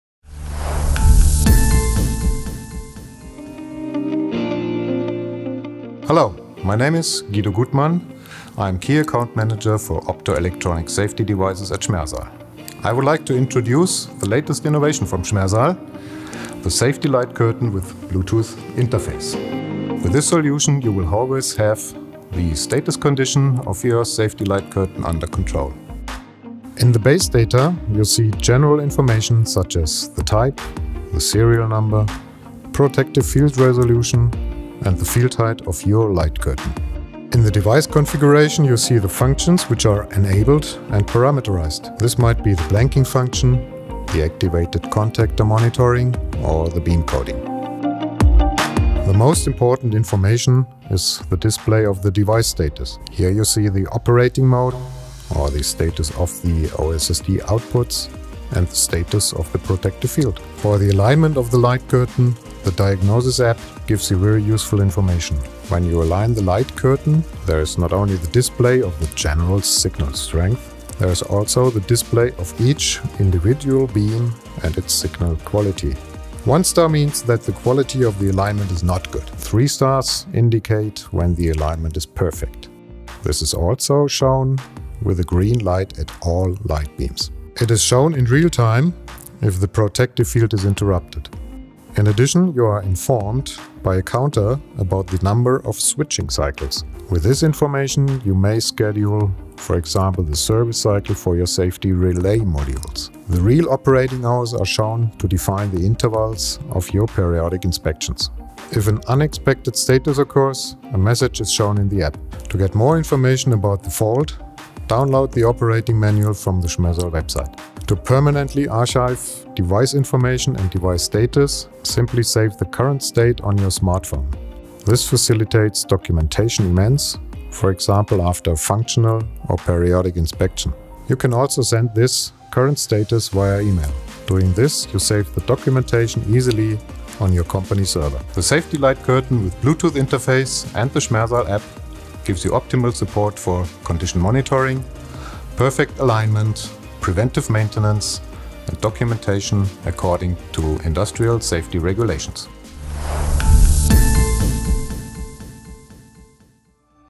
Safety light grid SLC440/COM with Bluetooth interface and APP
Guido Gutmann, Key Account Manager, Optoelectronic Safety Devices, Schmersal Group, explains the advantages of this new optoelectronic safety system that makes all diagnostic data of the device available on smartphones or tablets.